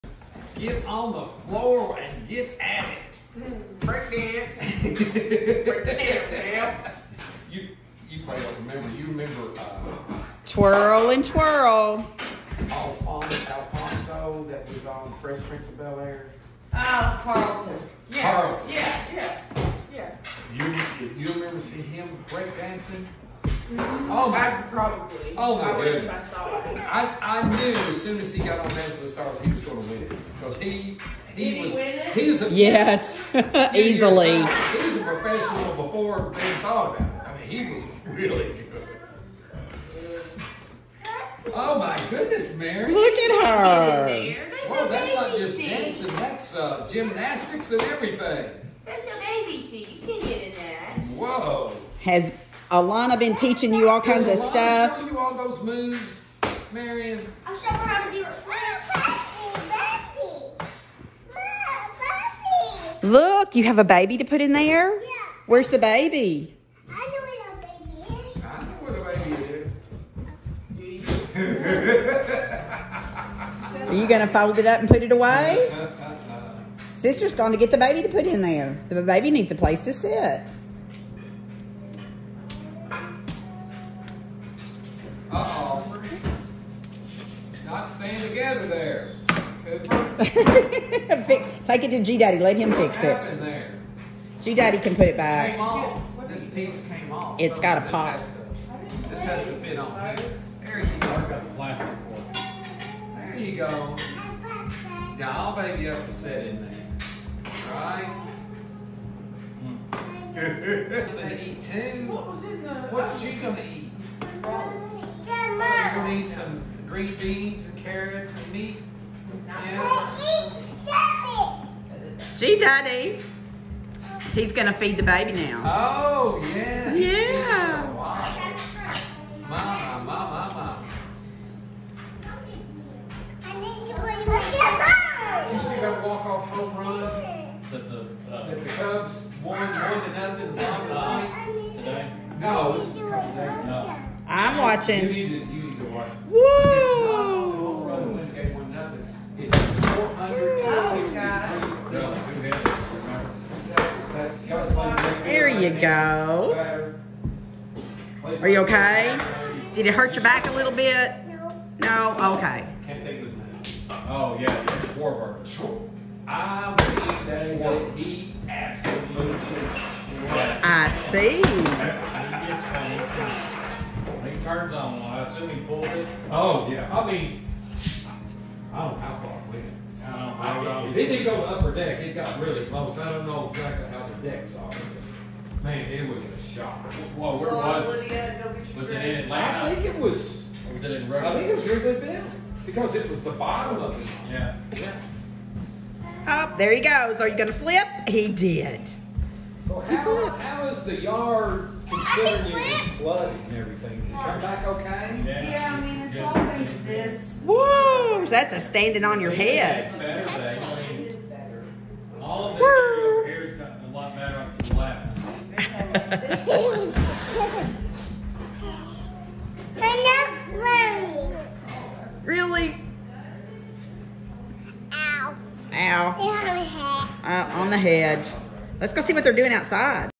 Marion & Cooper Showing off Their moves for Gigi & GDaddy 4/2021
Marion & Cooper Showing off Their moves for Gigi & GDaddy April 2021